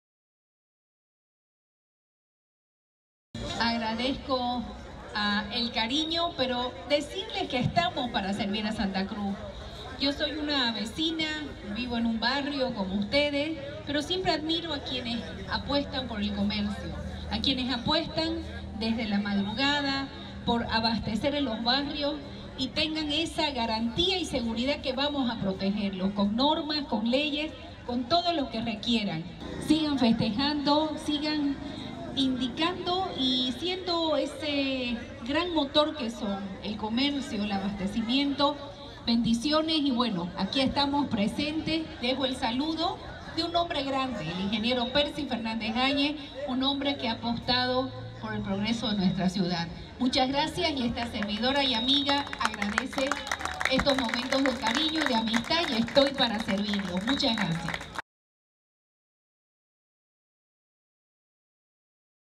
Presidenta del Concejo comparte con comerciantes del mercado San Juan
La presidenta del Concejo Municipal, Angélica Sosa, compartió con los comerciantes del mercado San Juan,  ubicado en la zona de la Villa Primero de Mayo, que estuvo de aniversario y les agradeció por su cariño, por su amistad y reiteró su compromiso de servirlos y continuar brindandoles el apoyo del Gobierno Municipal. Asimismo,hizo llegar  el saludo del alcalde, Percy Fernández, "un hombre grande que ha apostado por el progreso de nuestra ciudad".